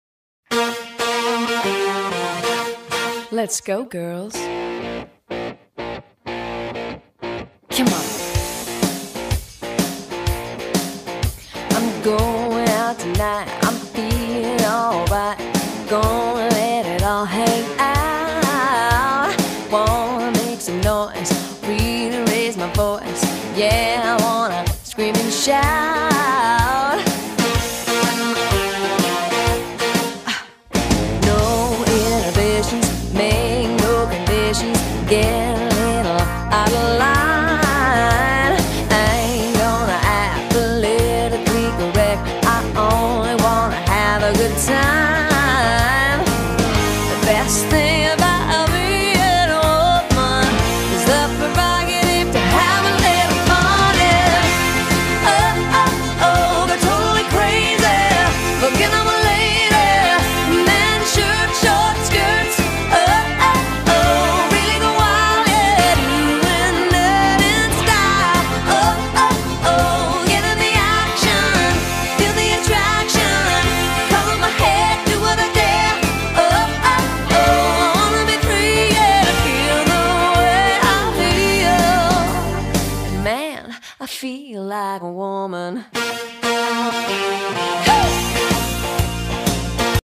desfile
el mejor desfile de señoritas pequeñas (esepto lin lin) una buena pasarela chequenlo esta bueno